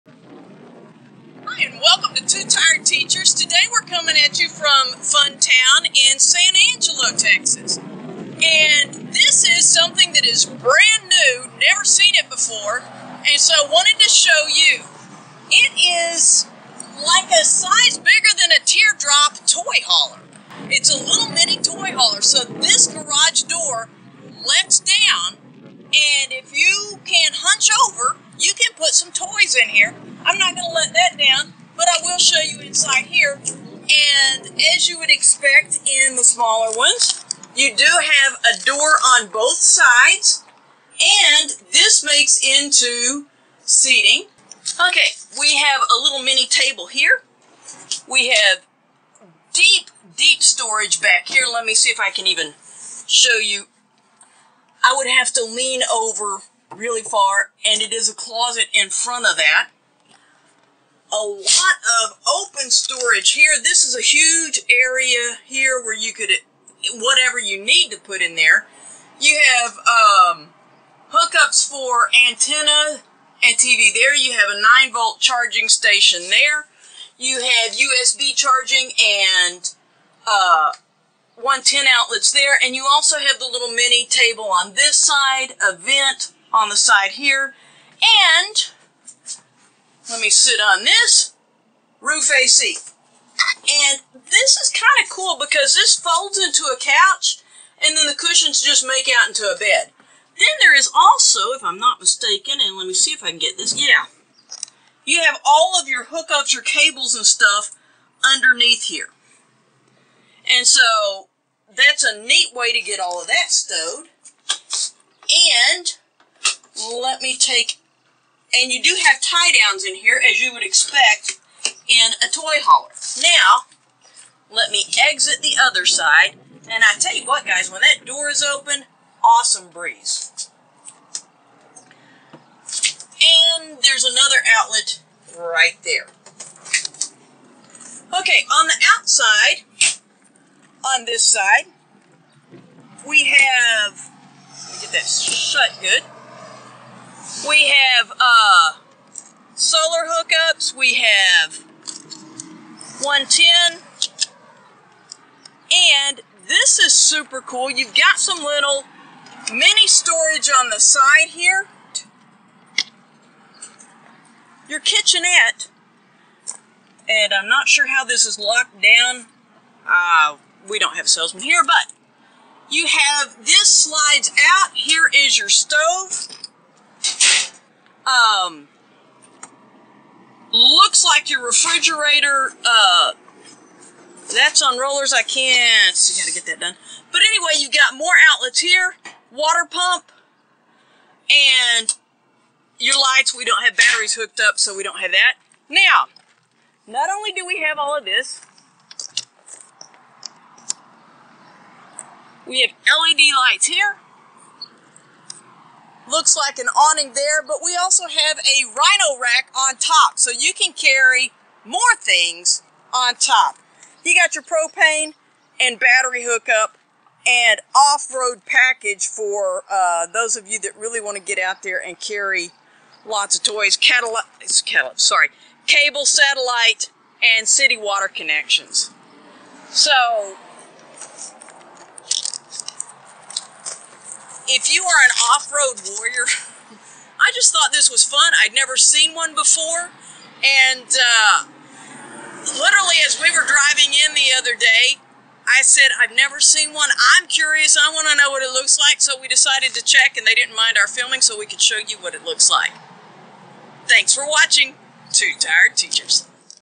No Bo Toy Hauler - No Boundaries Toy Hauler
No Bo Toyhauler

While visiting San Angelo, TX, the Two Tired Teachers went to Fun Town RV where we made this video of a toyhauler we had never before seen, the No Bo (No Boundaries) 10.6.

The No Bo has an incredible amount of storage for a unit that is like a slightly larger tear drop.  It has a wardrobe and small pantry.

It comes with a small outdoor kitchen with a refrigerator.  In addition, it has a rhino rack on top for additional storage.

One of the wonderful things that makers of tiny travel trailers has done is include loungers that make into beds.  It gives you seating and often a small tabletop.

The specs on the No Bo toyhauler are the exterior length is 13'9", width is 88" and height is 7'5".  It has a 30 gallon freshwater tank, and it weighs a mere 1606 pounds.

No Boundaries packs a lot into this small unit.